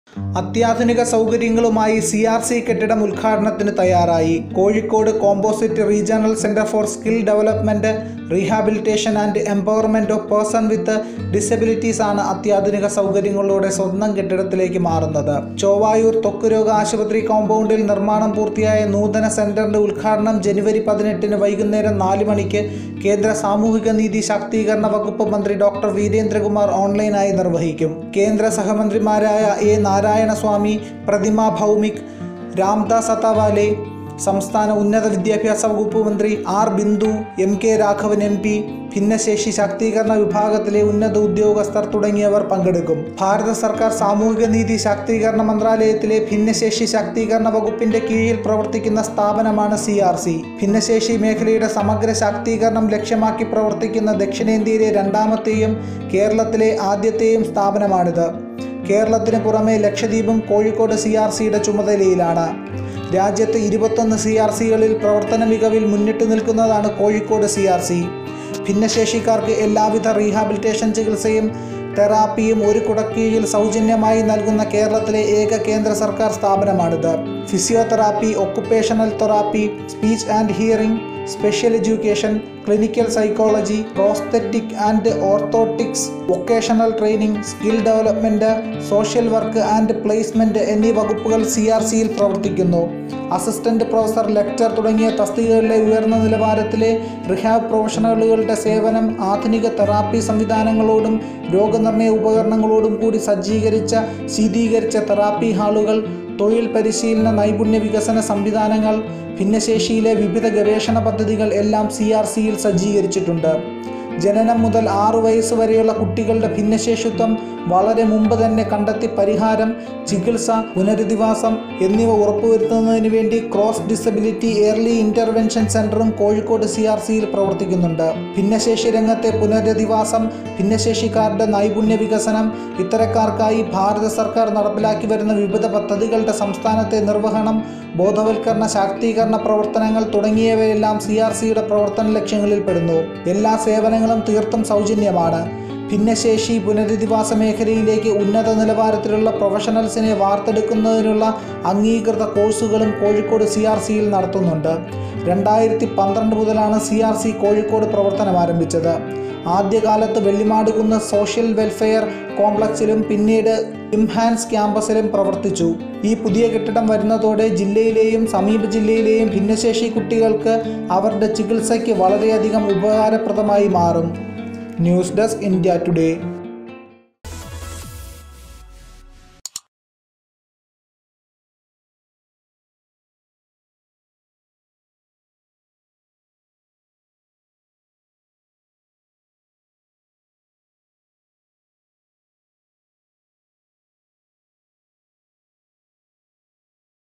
ഭിന്നശേഷിക്കാര്‍ക്ക് പ്രതീക്ഷകളുയര്‍ത്തി സിആര്‍സി ആസ്ഥാനം ഉത്ഘാടനത്തിനൊരുങ്ങി | Indiatoday Malayalam
compositeregionalcenterkozhikode compositeregionalcentresforpersonwithdisabilities(crcs) disabilityrehabilitationmalayalamvideos കോഴിക്കോട്: ആധുനിക സൗകര്യങ്ങളുമായി സിആര്‍സി കെട്ടിടം ഉല്‍ഘാടനത്തിനു തയ്യാറായി. കോഴിക്കോട് കോമ്പോസിറ്റ് റീജ്യണല്‍ സെന്റര്‍ ഫോര്‍ സ്‌കില്‍ ഡെവലപ്‌മെന്റ്, റീഹാബിലിറ്റേഷന്‍ & എംപവര്‍മെന്റ് ഓഫ് പേഴ്‌സണ്‍സ് വിത്ത് ഡിസബിലിറ്റീസ് ആണ് അത്യാധുനിക സൗകര്യങ്ങളോടെ സ്വന്തം കെട്ടിടത്തിലേക്ക് മാറുന്നത്. ചേവായൂര്‍ ത്വക്ക് രോഗആശുപത്രി കോമ്പൗണ്ടില്‍ നിര്‍മാണം പൂര്‍ത്തിയായ നൂതന സെന്ററിന്റെ ഉദ്ഘാടനം ജനുവരി 18 ന് വൈകുന്നേരം 4 മണിക്ക് കേന്ദ്ര സാമൂഹിക നീതി ശാക്തീകരണ വകുപ്പ് മന്ത്രി ഡോ. വീരേന്ദ്ര കുമാര്‍ ഓണ്‍ലൈനായി നിര്‍വ്വഹിക്കും. കേന്ദ്ര സഹമന്ത്രിമാരായ എ. നാരായണസ്വാമി, പ്രതിമ ഭൗമിക്, രാംദാസ് അതാവാലെ, സംസ്ഥാന ഉന്നത വിദ്യാഭ്യാസ വകുപ്പു മന്ത്രി ആര്‍. ബിന്ദു, എം.കെ. രാഘവന്‍ എംപി, ഭിന്നശേഷി ശാക്തീകരണ വിഭാഗത്തിലെ ഉന്നത ഉദ്യോഗസ്ഥര്‍ തുടങ്ങിയവര്‍ പങ്കെടുക്കും.